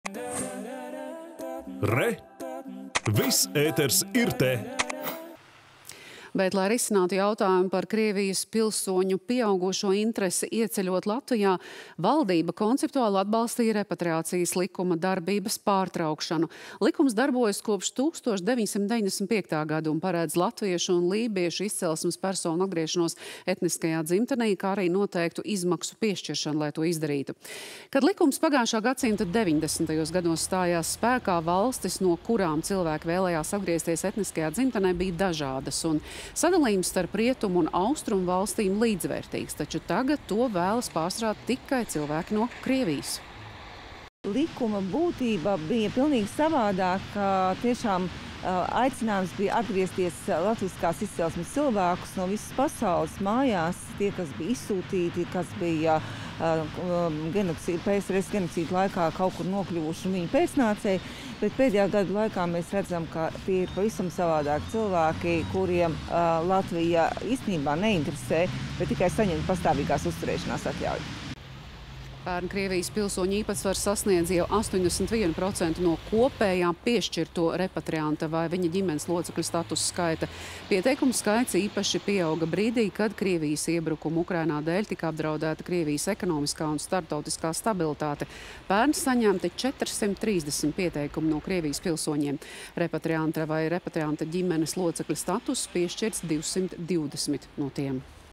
Plāno pārtraukt repatriācijas likuma darbību
Lai risinātu jautājumu par Krievijas pilsoņu pieaugošo interesi ieceļot Latvijā, valdība šodien konceptuāli atbalstīja Repatriācijas likuma darbības pārtraukšanu. Likums darbojas kopš 1995. gada un paredz latviešu un lībiešu izcelsmes personu atgriešanos etniskajā dzimtenē, kā arī noteiktu izmaksu nodrošināšanu šī procesa pilnvērtīgai veikšanai.